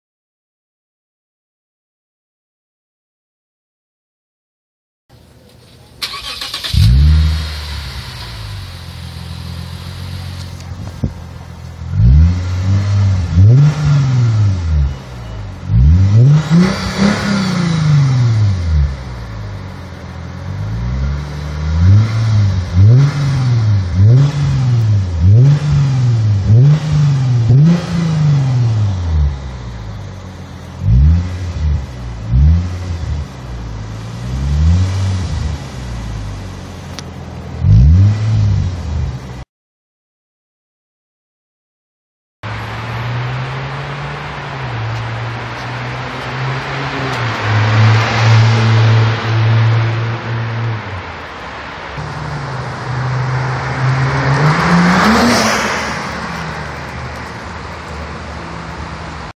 Tsudo Lexus IS250 IS350 2006-2010 SP2 Dual Axle-back Exhaust System
Tsudo Lexus IS250 IS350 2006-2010 SP2 Dual Axle-back Exhaust System. Sorry the beginning is a bit shaky